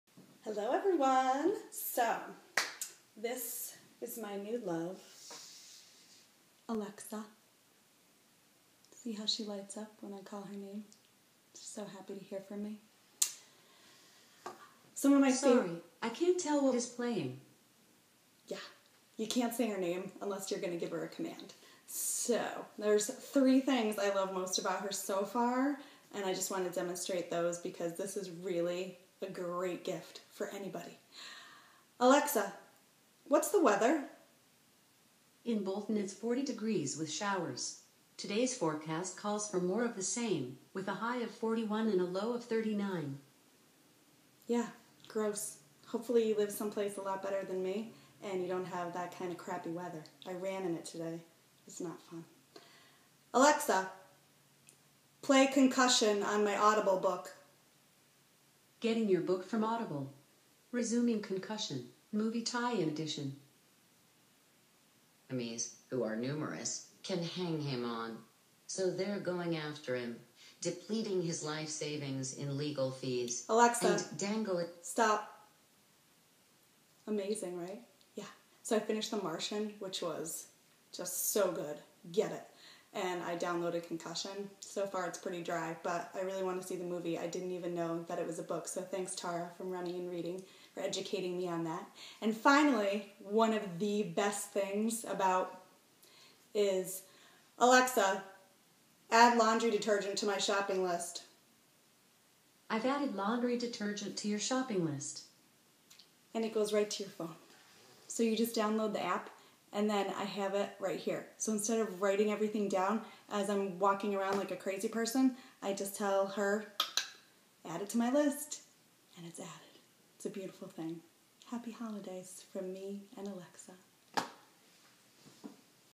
Amazon ECHO - My Three Favorite Things!
Why Alexa is the perfect gift for just about anyone and the top three ways I use her.